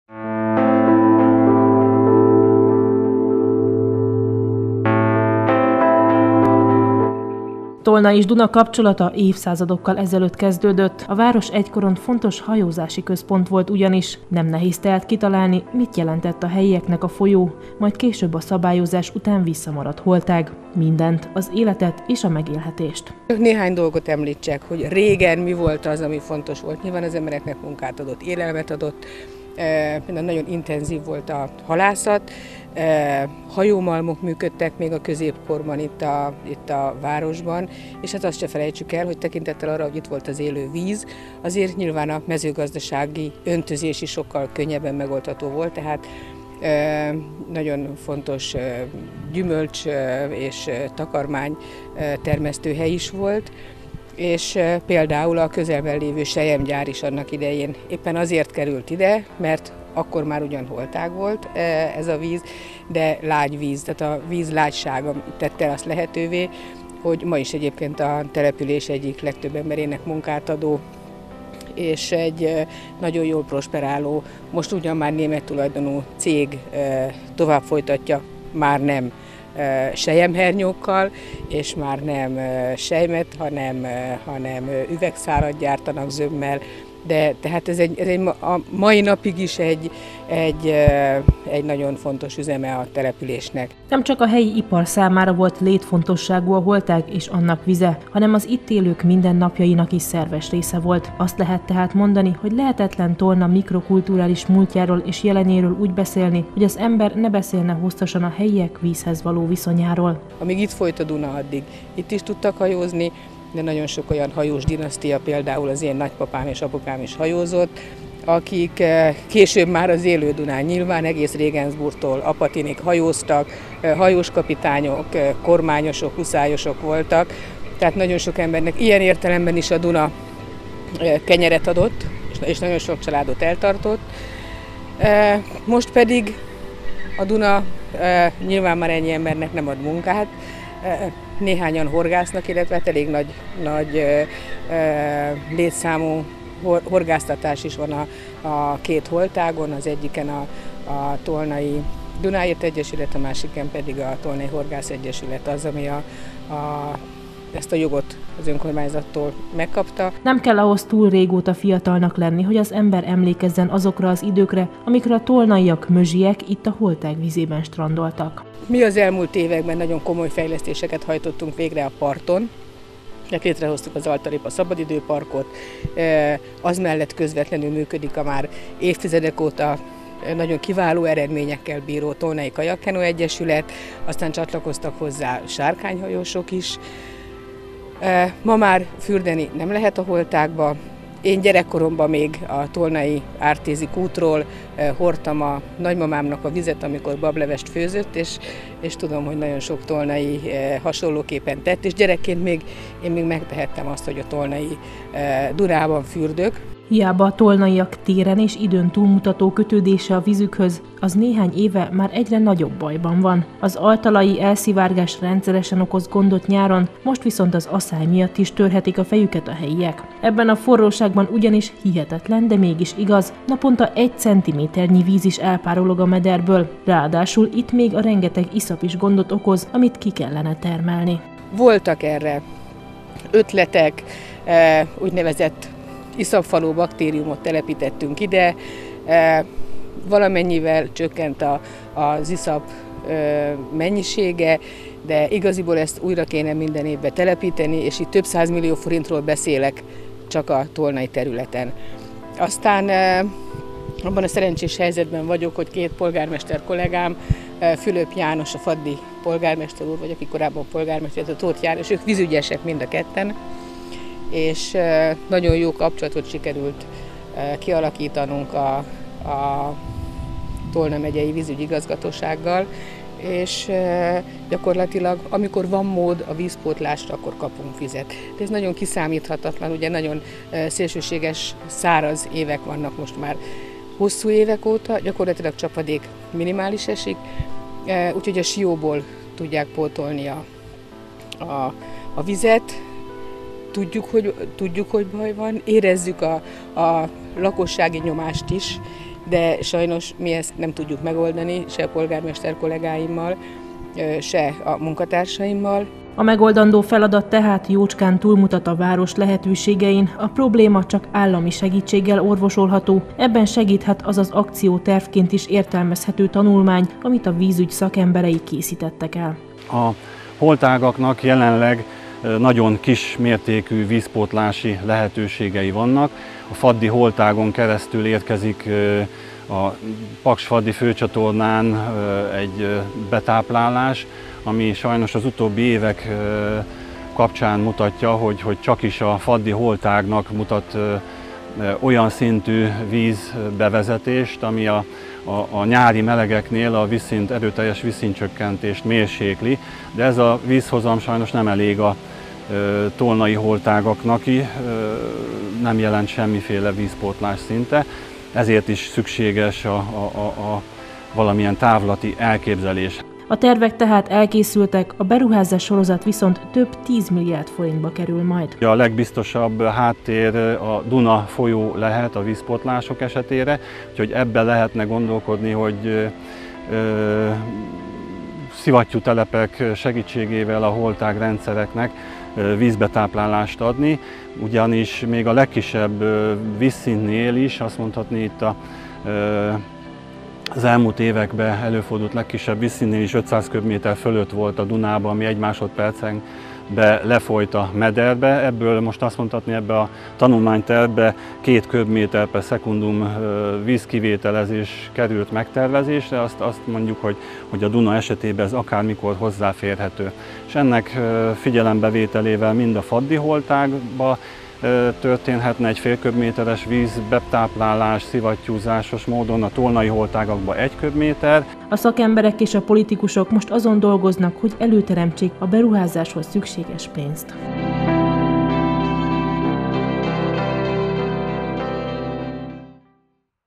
Komoly vízhiánnyal küzd a tolnai Duna holtág
Komoly vízhiánnyal küzd a tolnai Duna holtág. Ez szinte már általános jelenség az aszály miatt idehaza. Itt viszont még az nehezíti a dolgot, hogy túl sok az iszap a meder alján. A probléma megoldásához szükséges munka tervei már elkészültek, most forrásszerzésre koncentrálnak a szakemberek.